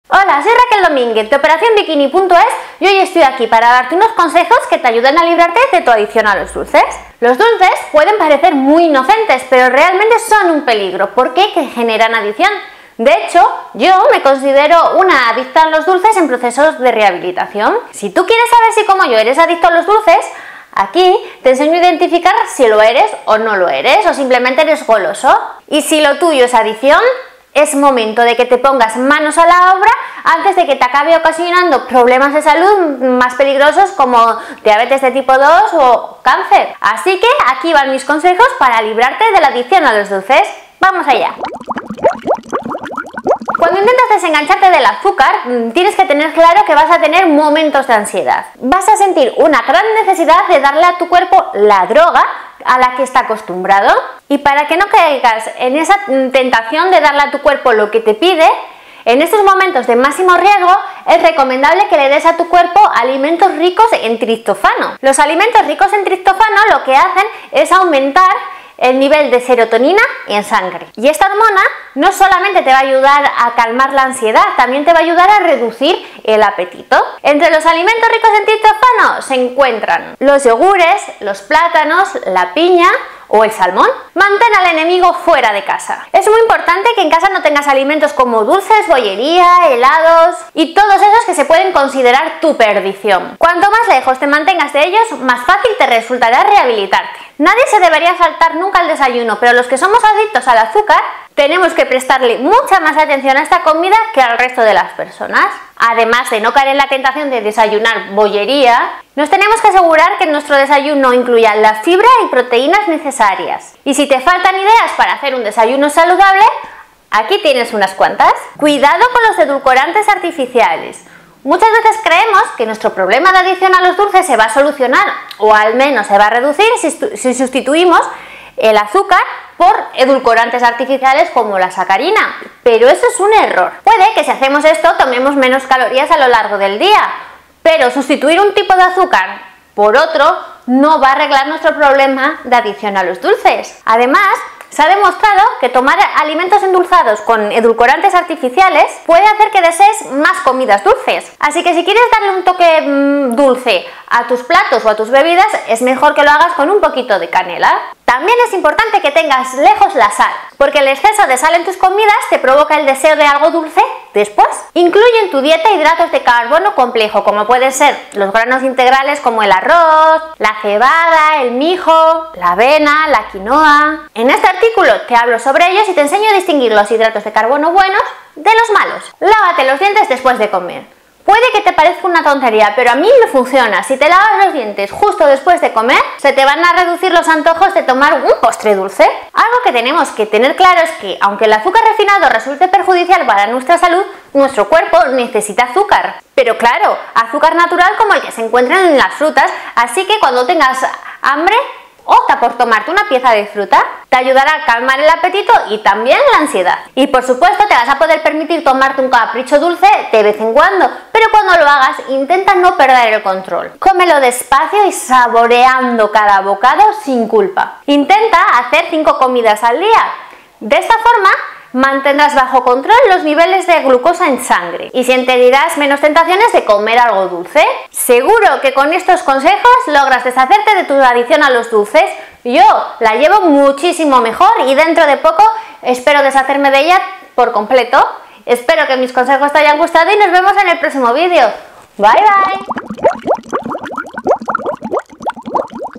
Como superar la adicción a los dulces
Los dulces pueden parecer muy inocentes pero son realmente peligrosos porque crean adicción, yo me considero una adicta a los dulces en proceso de rehabilitación, por eso hoy te quiero contar como superar la adicción a los dulces 😛

Si tú quieres saber si como yo, eres adicto a los dulces, en este artículo ya te enseñé a identificar si solo eres un poco goloso o si realmente tienes adicción, y si lo tuyo es adicción… es el momento de que te pongas manos a la obra para acabar con ella, antes de que acabe provocándote problemas más serios de salud como diabetes de tipo 2 u otras enfermedades.

Así que para que acabes con tu adicción a los dulces aquí tienes unos consejos para desengancharte del azúcar.

10 Consejos para superar la adicción a los dulces:

Cuando intentas desengancharte del azúcar tienes que tener claro que vas a tener momentos de ansiedad, vas a sentir una gran necesidad de darle a tu cuerpo “su droga” y para que no caigas va a tener que darle a tu cuerpo en ese momento de “máximo riesgo” alimentos ricos en triptófano que favorecen la elevación de serotonina en sangre, esta hormona no solo te va a ayudar a reducir la ansiedad también te ayudará a calmar el apetito, entre los alimentos ricos en triptófano se encuentran los yogures, plátanos, piña o salmón.
Mantén al enemigo fuera de casa, es muy importante que en casa no tengas alimentos como dulces, bollería, helados… y todos esos que se pueden considerar tu perdición, cuanto más lejos te mantengas de ellos más fácil te resultará “rehabilitarte”
Nadie se debería saltar nunca el desayuno, pero los que somos adictos a los dulces tenemos que tener especial cuidado en la hora del desayuno, además de no caer en la tentación de incluir bollería en él nos tenemos que asegurar de que incluye la fibra y las proteínas necesarias, si te faltan ideas para hacer un desayuno saludable aquí tienes unas cuantas.
Cuidado con los edulcorantes artificiales, muchas veces creemos que nuestro problema de adicción al azúcar se resuelve (o por lo menos se alivia) sustituyendo el azúcar por edulcorantes como la sacarina, pero creer eso es sólo engañarnos a nosotros mismos, puede que así tomemos menos calorías a lo largo del día pero sustituir azúcar con otro tipo de azúcar no sanará la adicción en sí. Además se ha demostrado que tomar alimentos endulzados con edulcorantes artificiales puede hacer que desees comer más comidas dulces, si quieres darle un toque dulce a tus platos o a tus bebidas mejor que lo hagas con canela.
También tienes que procurar tener lejos la sal, el exceso de sal en tus comidas te crea deseo de dulce después.
Incluye en tu dieta hidratos de carbono complejos como granos integrales como arroz, cebada, mijo, avena, quinoa… en este artículo te hablo más sobre ellos y te enseño a distinguir los hidratos de carbono buenos de los malos.
Lávate los dientes después de comer, no esperes, puede que te parezca una tontería pero a mi me ayuda a reducir los antojos de un “postre dulce” al terminar de comer.
Aunque el azúcar refinado es un problema para nuestro cuerpo necesitamos azúcar, pero claro azúcar natural como el que podemos encontrar en las frutas, así que cuando tengas hambre toma fruta, te ayudará a calmar el apetito también y la ansiedad.
Puedes permitirte un capricho de vez en cuando, pero cuando lo hagas intenta no perder el control, cómelos despacio y saboreando cada bocado sin culpa. Un capricho de vez en cuando no te va a impedir superar la adicción a los dulces :)
Intenta hacer 5 comidas al día, de esta manera mantendrás bajo control los picos de glucosa en sangre y sentirás menos tentaciones de comer dulces.
Espero que pongas cuanto antes en marcha estos consejos para superar la adicción a los dulces, puede que al principio te cueste un poco, pero sinceramente yo creía que mi vida sin dulces iba a ser mucho más dura :)